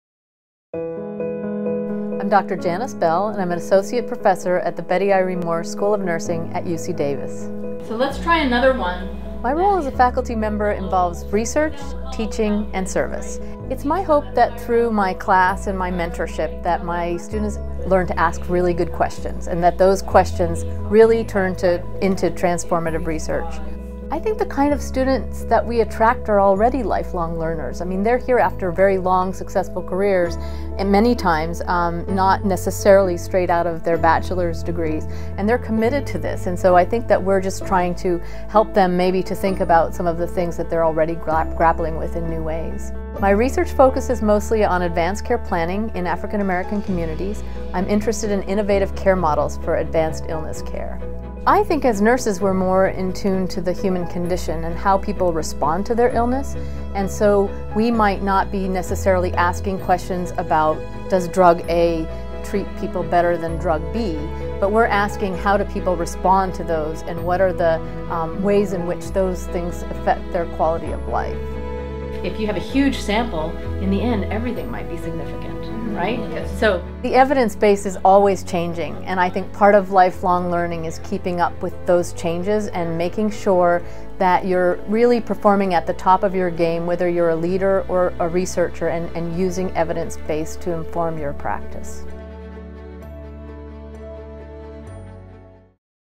UC Davis nursing professor discusses value of lifelong learning
Associate Professor Janice Bell discusses her view on lifelong learning and how that quest for knowledge translates to graduate students at the Betty Irene Moore School of Nursing at UC Davis. The conversation is part of UC Davis Health's "Year of Learning" launched for the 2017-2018 academic year.